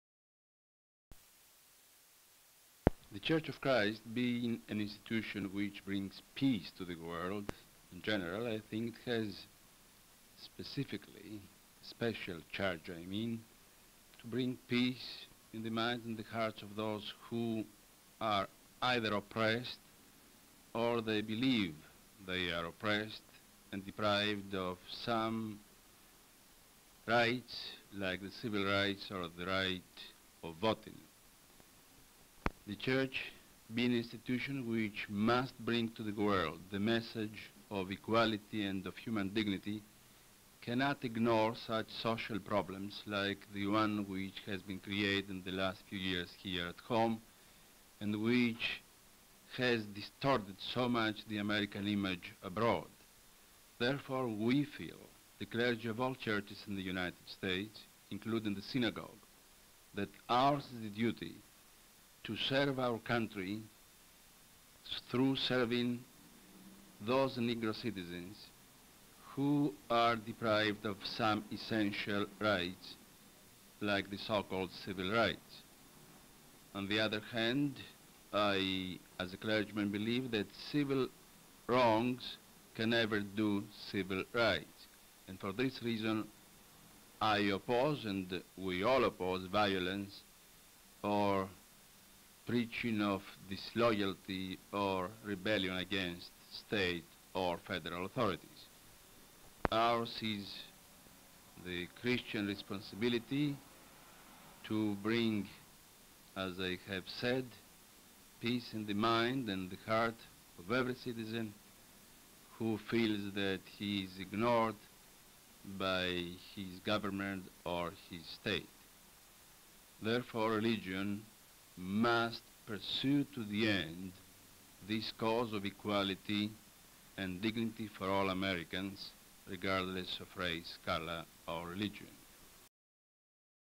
WIS 65 265
Archbishop Iakovos of America at Clergy Laity Congress, Columbia, SC, March 22nd 1965.  Archbishop Iakovos responses to WIS TV News regarding march with Martin Luther King in Selma, Alabama.